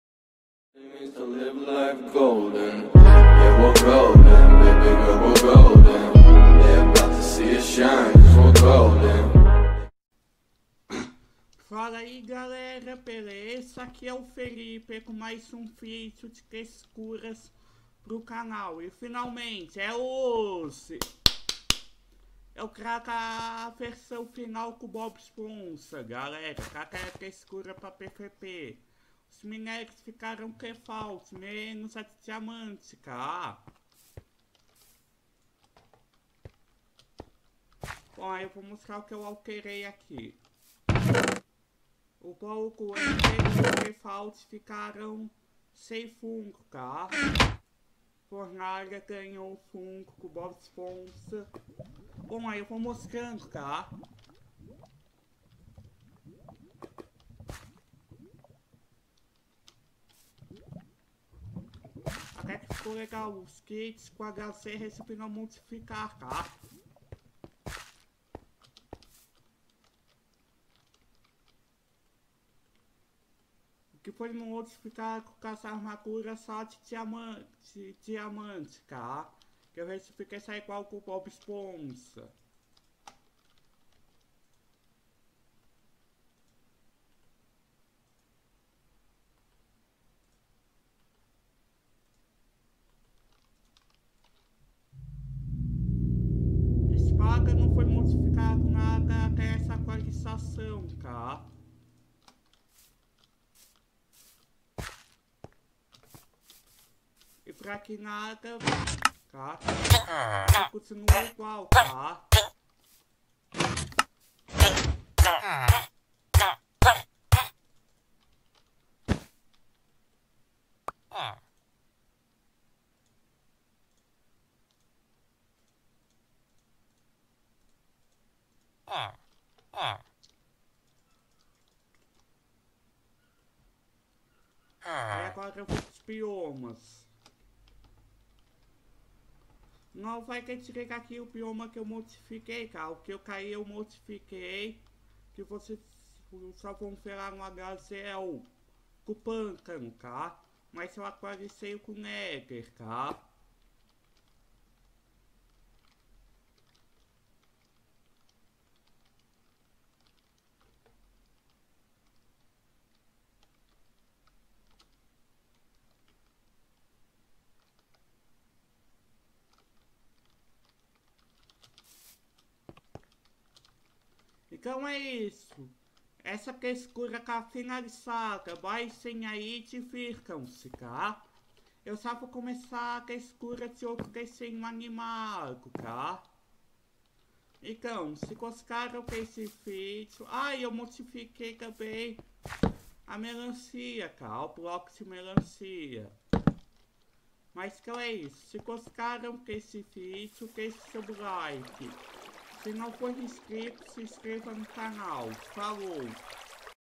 Textura do Bob Esponja Versão Final para pvp e HG #5 Texturas
▬▬▬▬▬▬▬▬▬ஜ۩۞۩ஜ▬▬▬▬▬▬▬▬▬▬
Mais Informações Logo a Baixo
▬▬▬▬▬▬▬▬▬ஜ۩۞۩ஜ▬▬▬▬▬▬▬▬▬▬
✪ Links e Downloads ✪
( se for postar no seu canal deixe meus créditos ou leva strike )
✪ Descrição ✪
Eu trago o lançamento da Textura do Bob esponja para pvp e HG Versão final , muitas coisas ficaram defaults mas tudo bem .